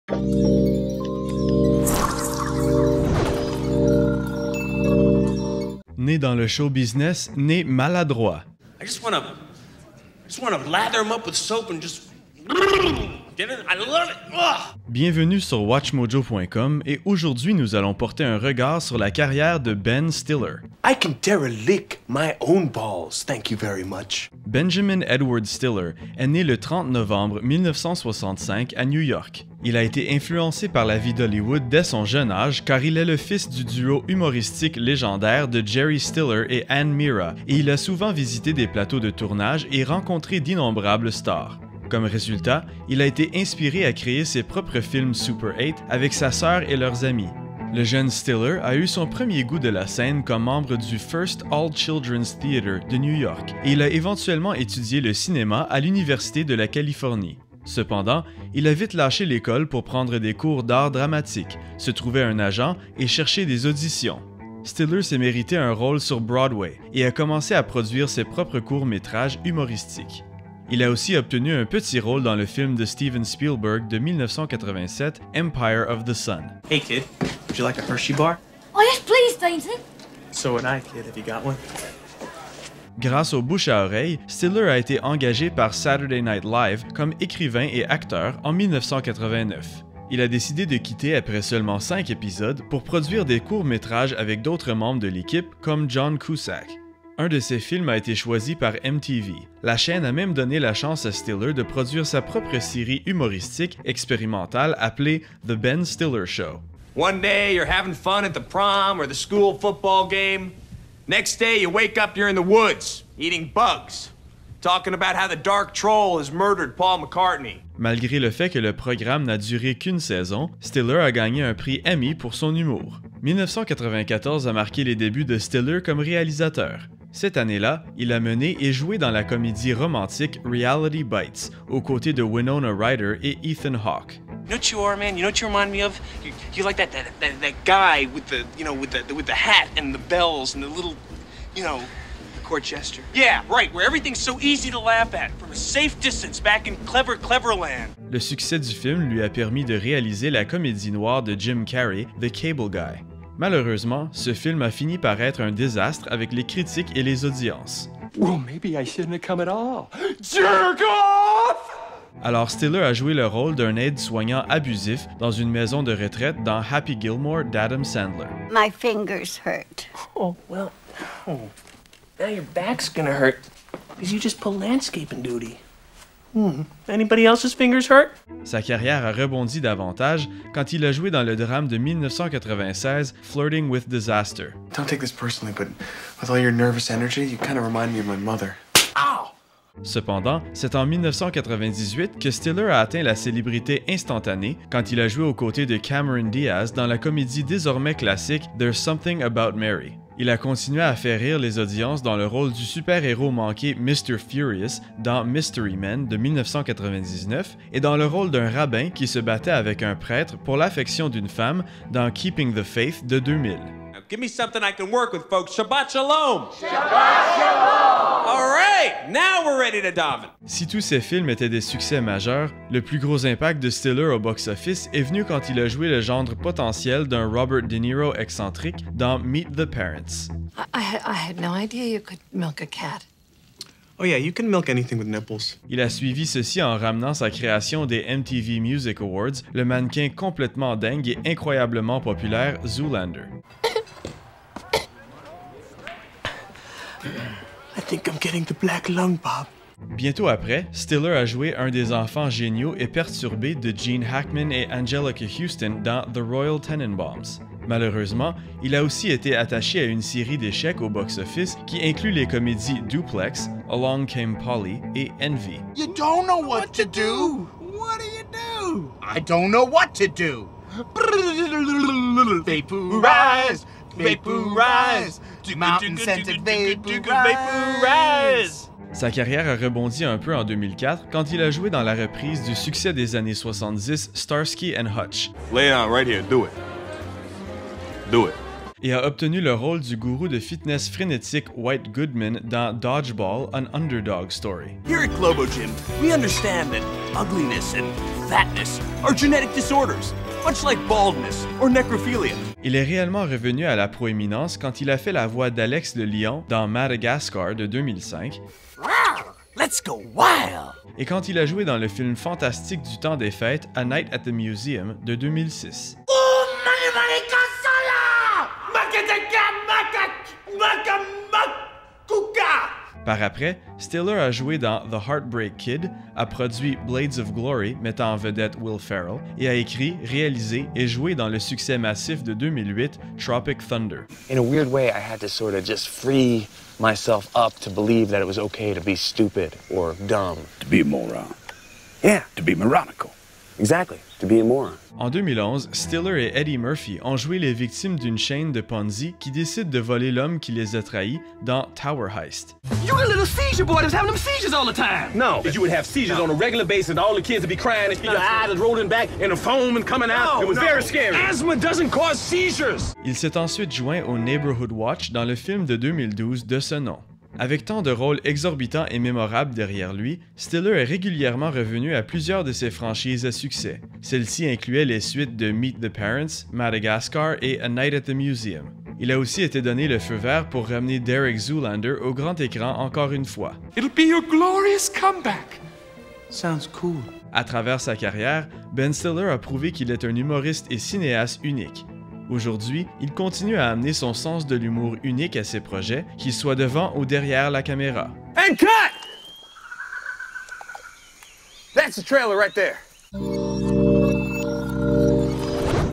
La Vie et la Carrière de Ben Stiller: De Zoolander à Tower Heist
Il est né dans le show business, et est devenu célèbre pour son sens de l'humour. Joignez-vous à en portant un regard sur la carrière de Ben Stiller.